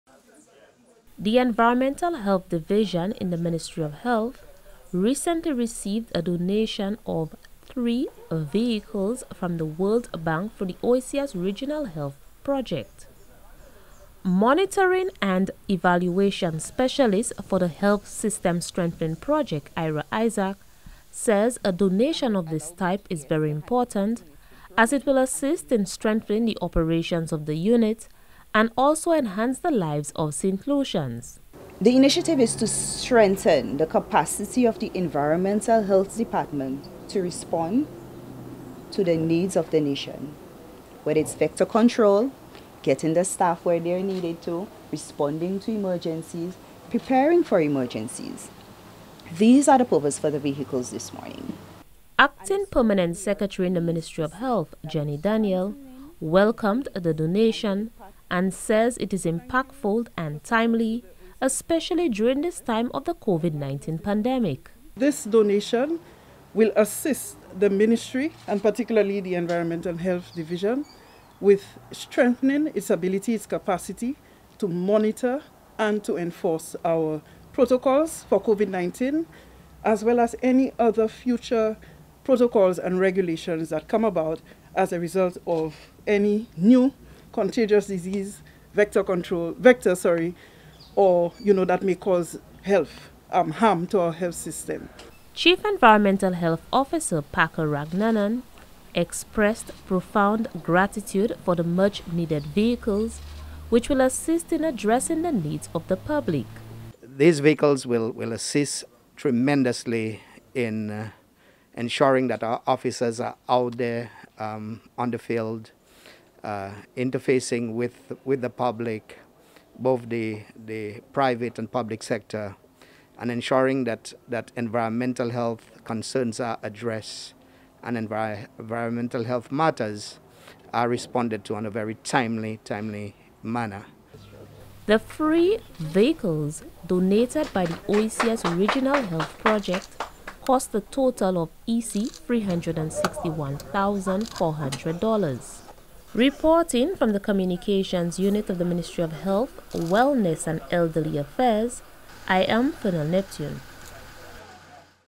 Division of Environmental Health receives vehicle donation from World Bank
The Environmental Health Division in the Ministry of Health recently received a donation of three vehicles from the World Bank through the OECS Regional Health Project.

Monitoring and Evaluation Specialist for the Health System Strengthening Project Ira Isaacs says a donation of this type is very important as it will assist in strengthening the operations of the unit and also enhance the lives of the Saint Lucian people.